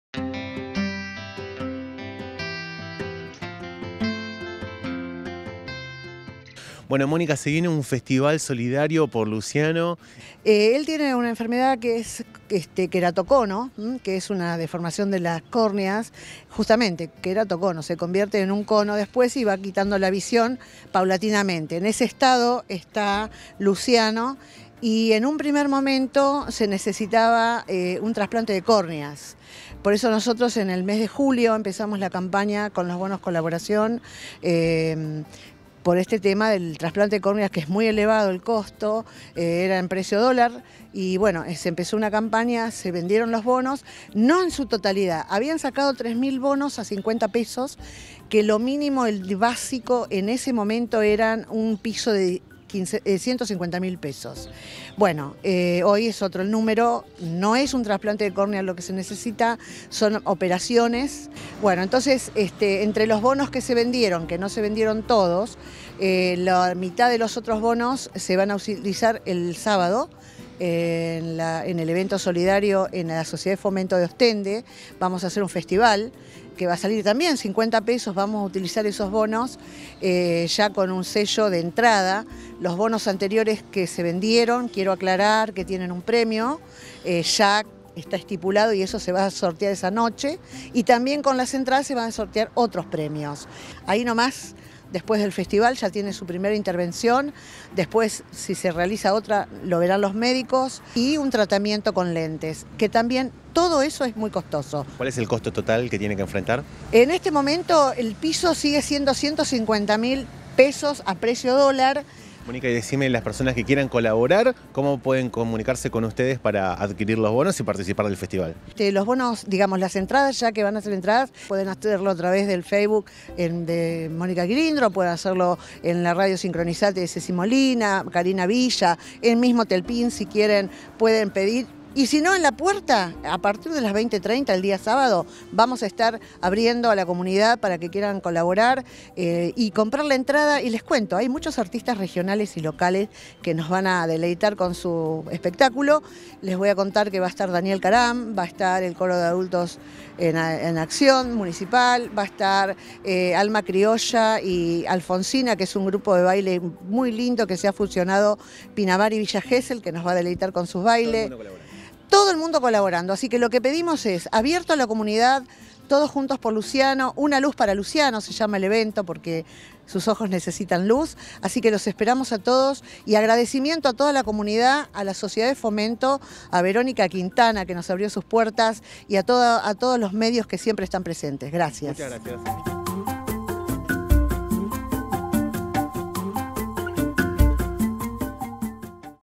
UNA LUZ PARA LUCIANO
Este fin de semana se llevará a cabo un festival solidario para ayudar a Luciano, un joven que debe afrontar con urgencia una operación en su vista. El testimonio de Mónico Quilindro, con todos los detalles.